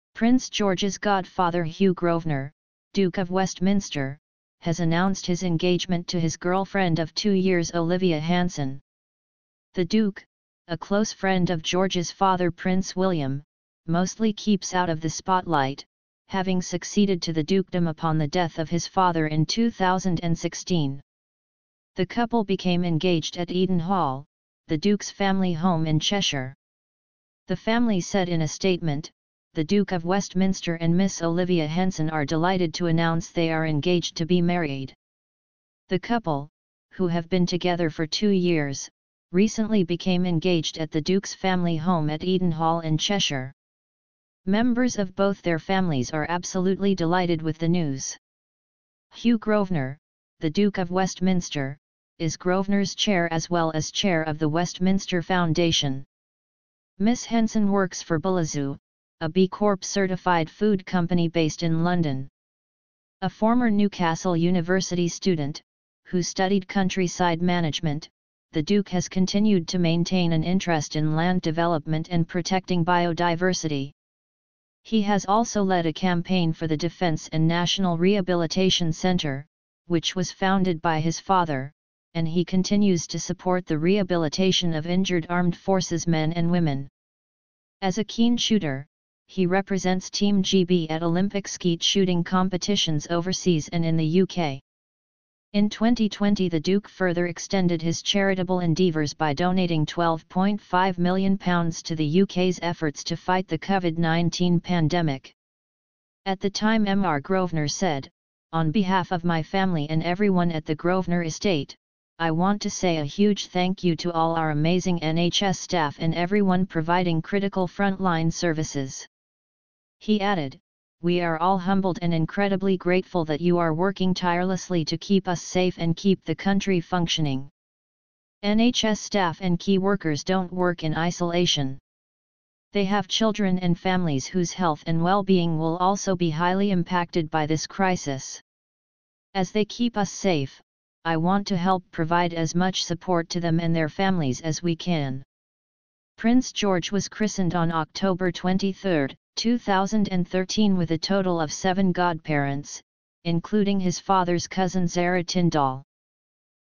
Prince George's godfather announces engagement to girlfriend of two years
Prince George's godfather Hugh Grosvenor, Duke of Westminster, has announced his engagement to his girlfriend of two years Olivia Hanson. The Duke, a close friend of George's father Prince William, mostly keeps out of the spotlight, having succeeded to the Dukedom upon the death of his father in 2016.

The couple became engaged at Eaton Hall, the Duke's family home in Cheshire.

The family said in a statement: "The Duke of Westminster and Miss Olivia Henson are delighted to announce they are engaged to be married.

"The couple, who have been together for two years, recently became engaged at the Duke's family home at Eaton Hall in Cheshire.

"Members of both their families are absolutely delighted with the news.

"Hugh Grosvenor, The Duke of Westminster, is Grosvenor's Chair as well as Chair of the Westminster Foundation.

"Miss Henson works for Belazu, a B-Corp certified food company based in London."

A former Newcastle University student, who studied countryside management, the Duke has continued to maintain an interest in land development and protecting biodiversity.

He has also led a campaign for the Defence and National Rehabilitation Centre, which was founded by his father, and he continues to support the rehabilitation of injured Armed Forces men and women.

As a keen shooter, he represents Team GB at Olympic Skeet Shooting competitions overseas and in the UK.

In 2020 the Duke further extended his charitable endeavours by donating £12.5million to the UK's efforts to fight the COVID-19 pandemic.

At the time Mr Grosvenor said: "On behalf of my family and everyone at the Grosvenor Estate, I want to say a huge thank you to all our amazing